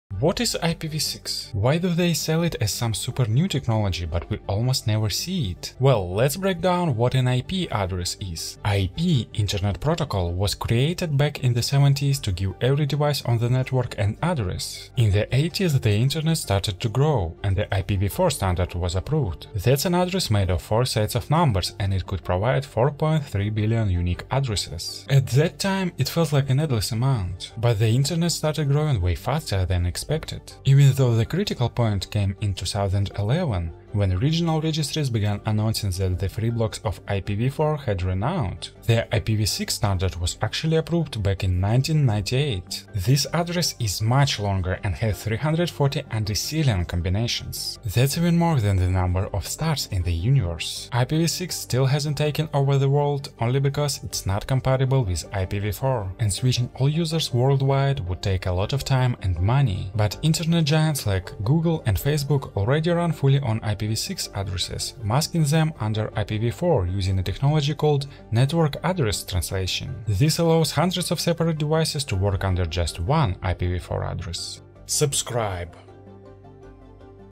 WHY DON'T WE USE IPv6? OR DO WE???
IP - Internet Protocol was created back in the 70s to give every device on the network an address. In the 80s, the internet started to grow and the IPv4 standard was approved. That’s an address made of four sets of numbers, and it could provide 4.3 billion unique addresses. At that time, it felt like an endless amount. But the internet started growing way faster than expected. Even though the critical point came in 2011, when regional registries began announcing that the free blocks of IPv4 had run out.